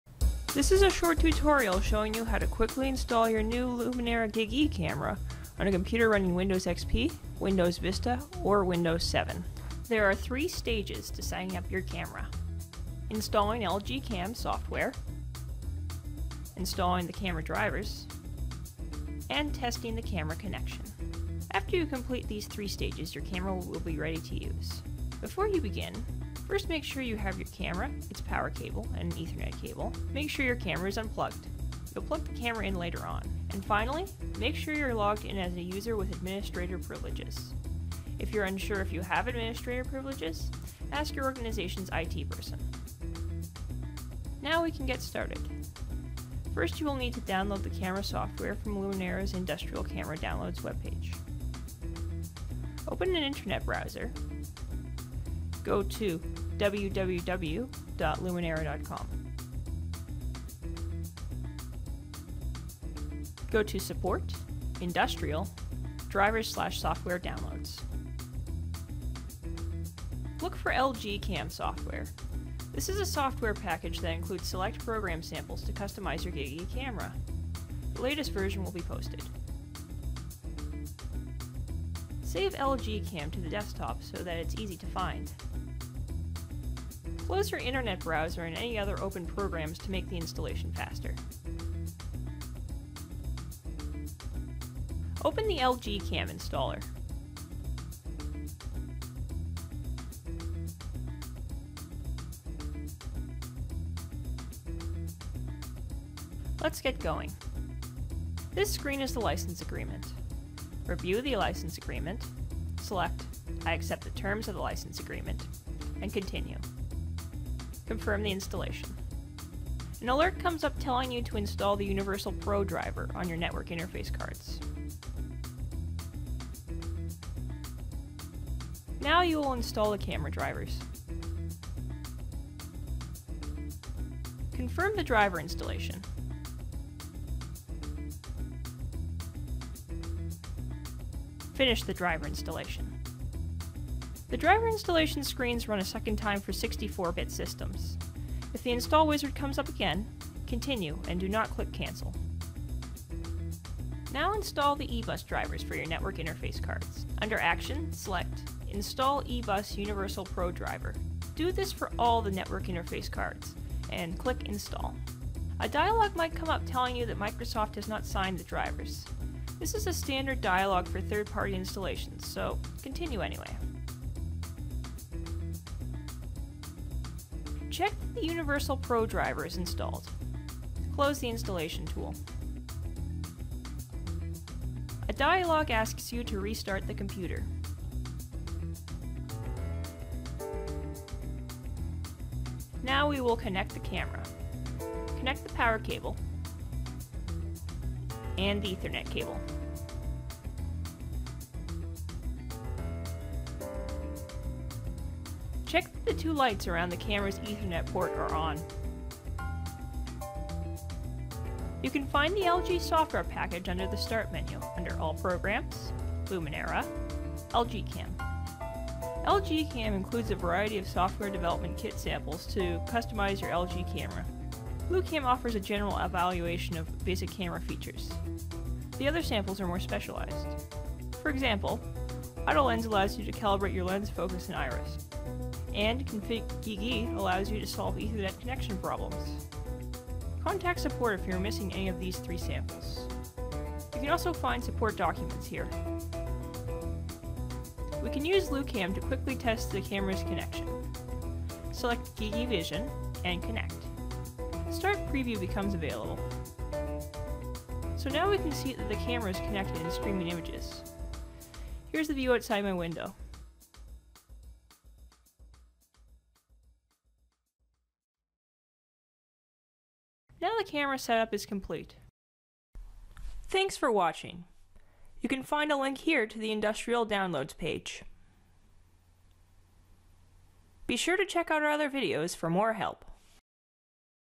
Installing Lumenera's LuCam Capture Software for a GigE Camera
This video provides instructions on installing the LuCam Software for a GigE camera on 32-bit or 64-bit computers running either Windows XP, VISTA, 7 or 8.